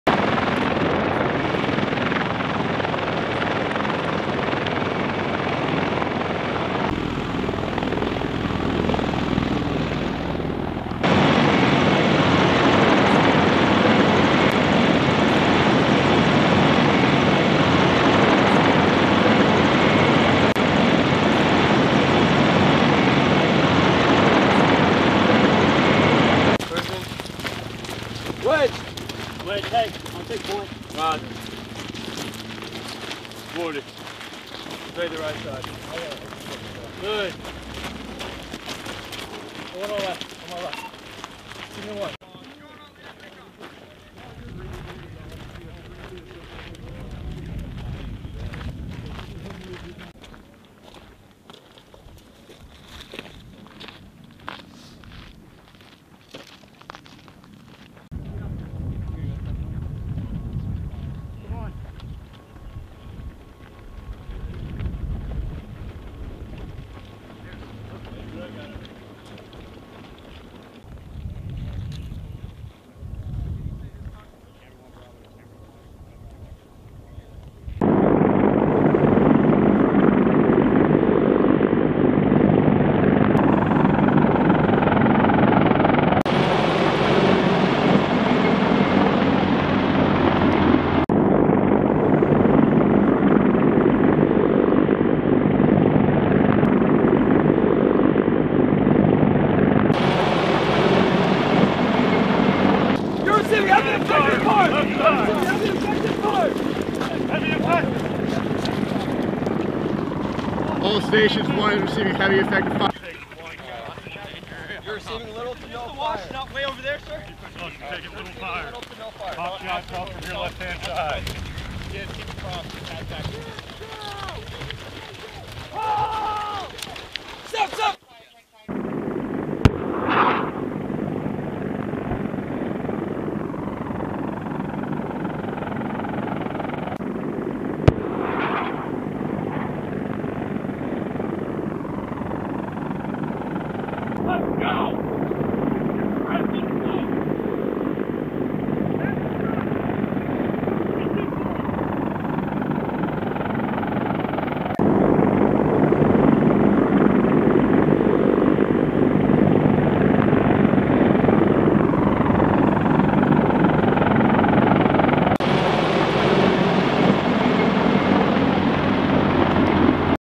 WTI 1-24: AST-2 assault raid
YUMA PROVING GROUND, AZ, UNITED STATES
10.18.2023
Video by Lance Cpl. Ruben Padilla

U.S. Marines assigned to Marine Aviation Weapons and Tactics Squadron One, participated in an assault raid exercise during Assault Support Tactics 2 (AST-2), part of Weapons and Tactics Instructors (WTI) course 1-24, at Yuma Training Range Complex, Arizona, Oct. 18, 2023. WTI is an advanced, graduate-level course for selected pilots and enlisted aircrew providing standardized advanced tactical training and assists in developing and employing aviation weapons and tactics. AST-2 provides prospective weapons and tactics instructors the opportunity to plan, brief, and execute a daytime, battalion-sized, live-fire air assault mission to multiple objective areas, while integrating combined arms and the six functions of Marine Aviation within an expeditionary advanced base operations scenario.